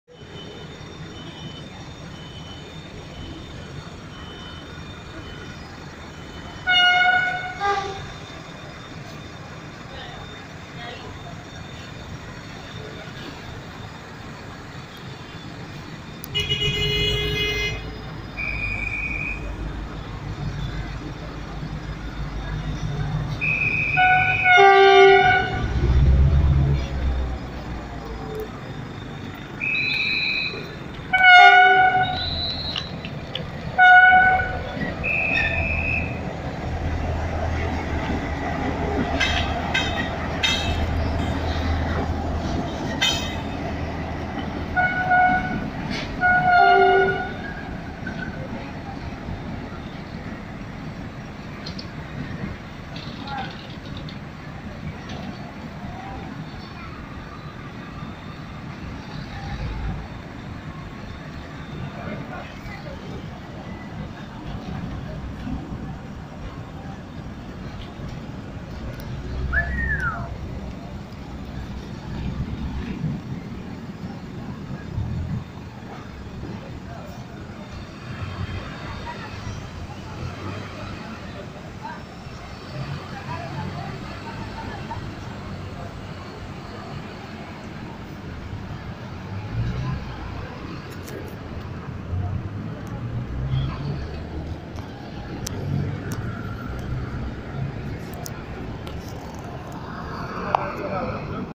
El Tren se Va Temprano
Un pueblo sin energía eléctrica. Como siempre, la ineficiencia y la codicia de las empresas privadas de servicios. El tren se va temprano, y su precio de boleto, un destello más de la depredación que reina.